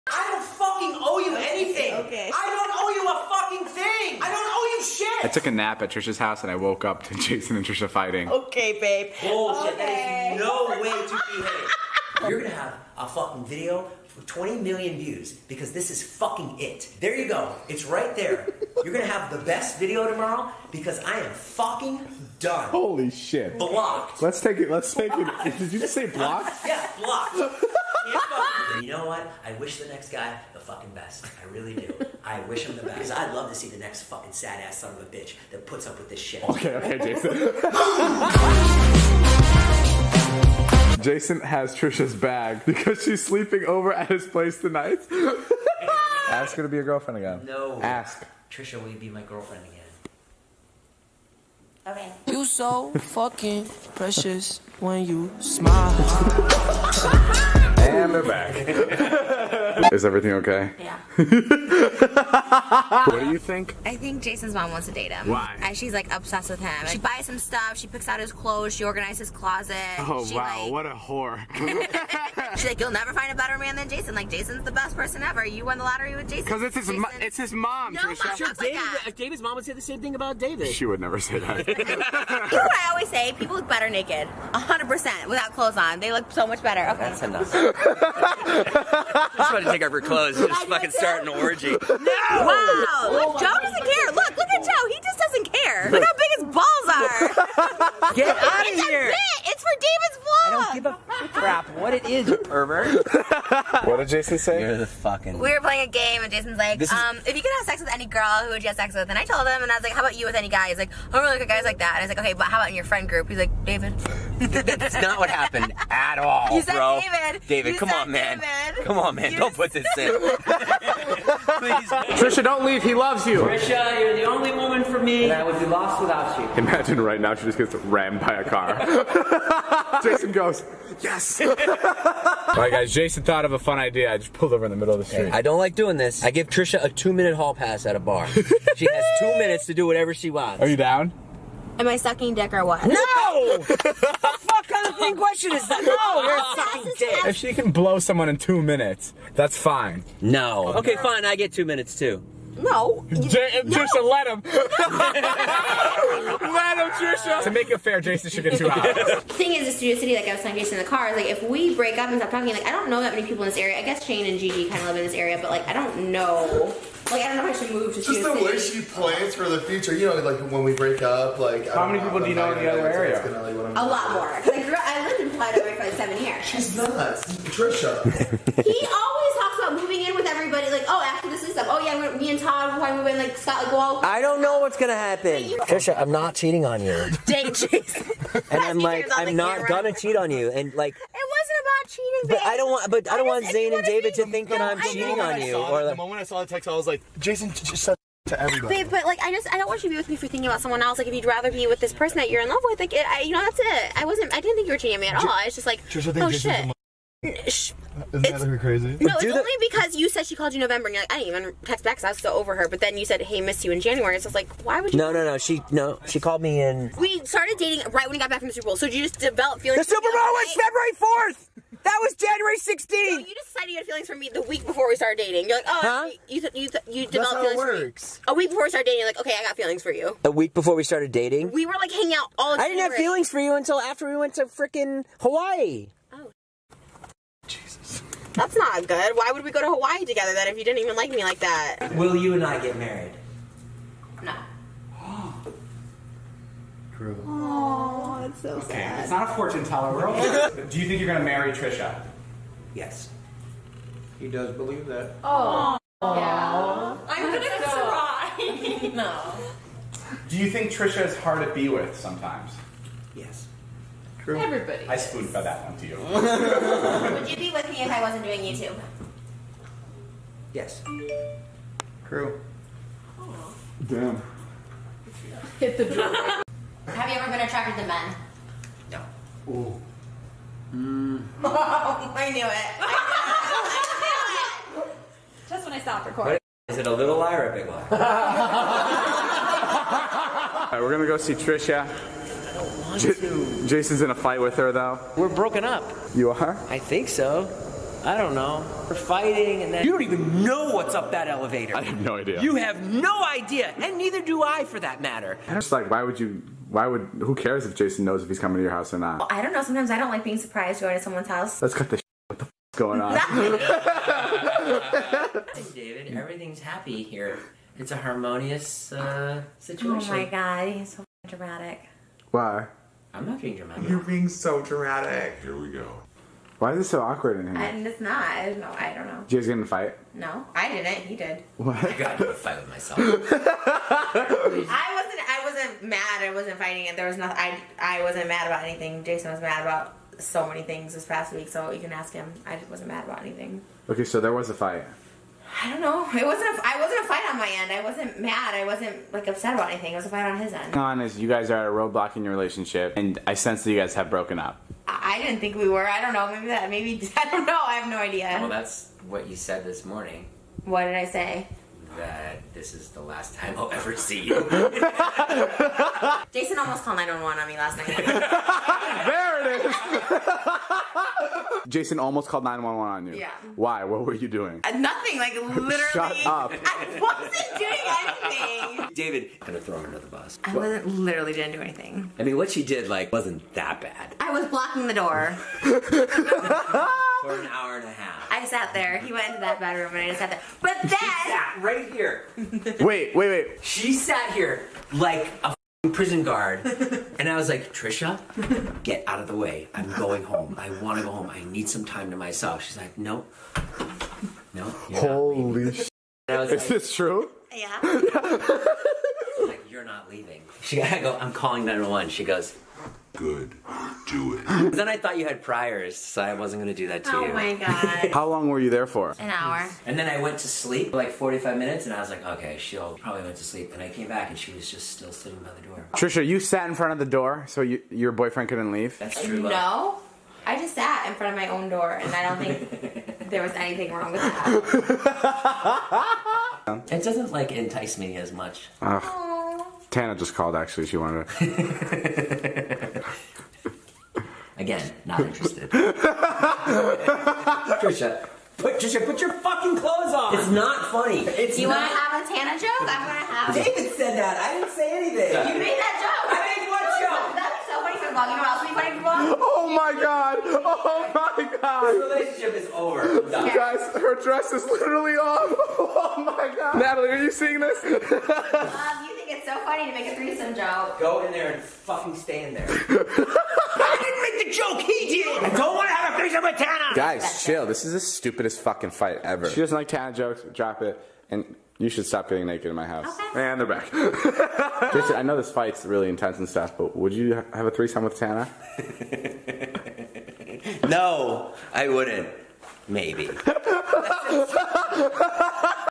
Trisha and Jason Breaking Up (Plus Extra Fights) Compilation
Video of Jason and Trisha Break ups/Fights

This video is not intended to hurt those involved it's just for entertainment.